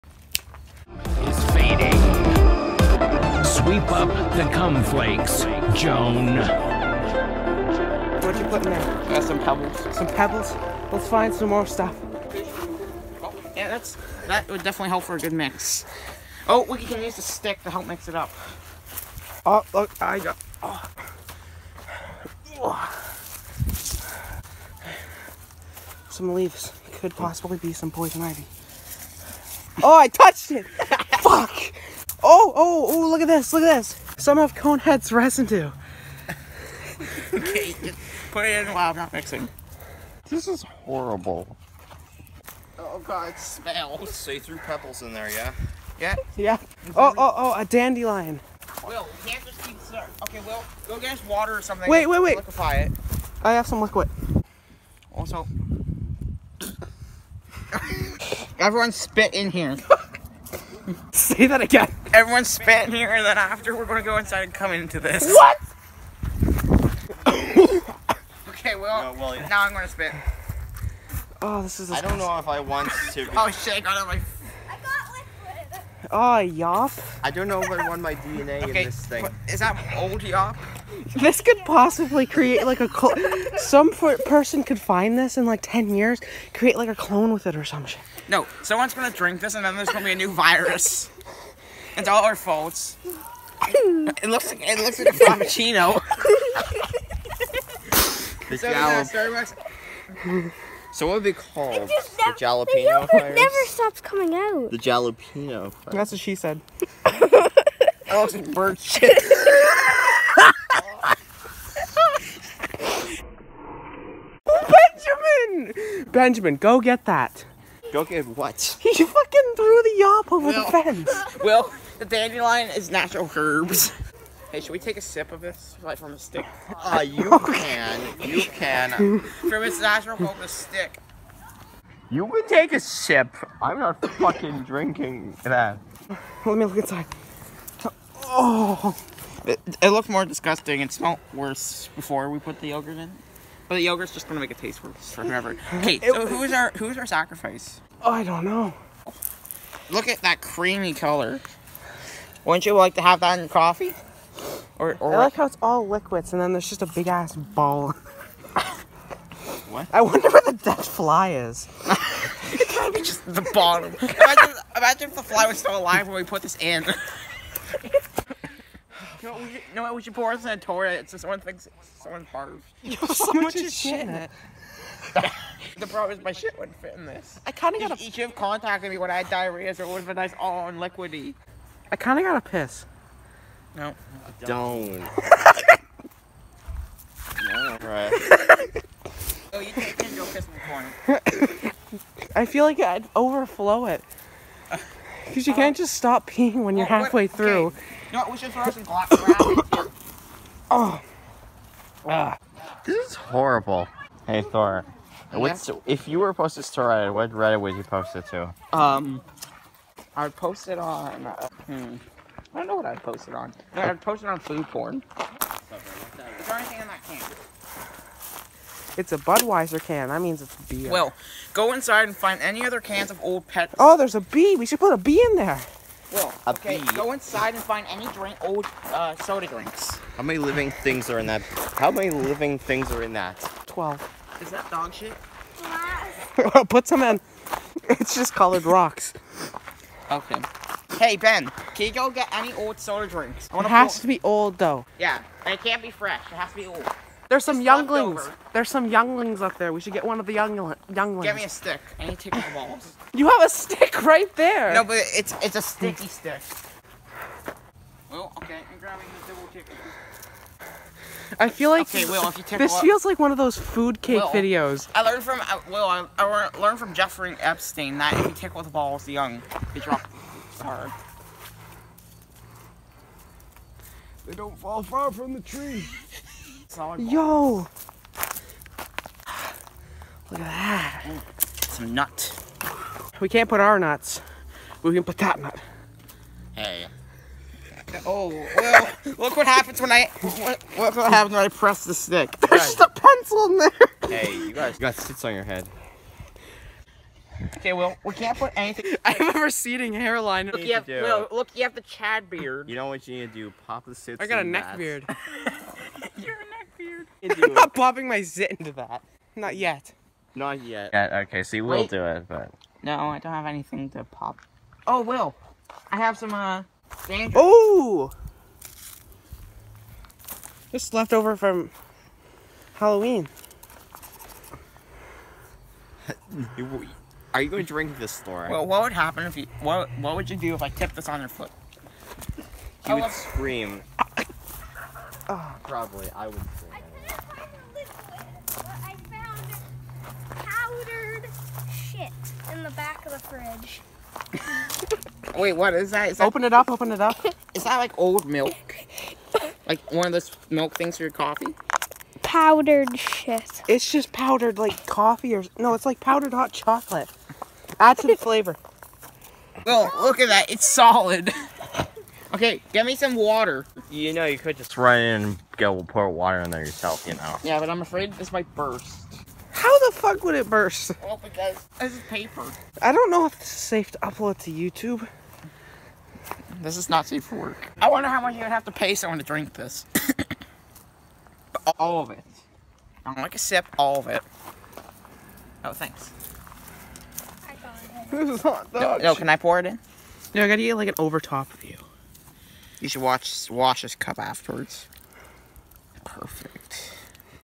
The Most Cursed Pringle’s Can Ever | Part 2: Jalapeño Frappuccino
The next part of “The Cursed Pringle’s Can” where we go outside and make it even worse! Part 3 will be out tomorrow!

All Music I Use in The Background Of My Videos Can be Found Here!

2
0
2
1

WHM
2021?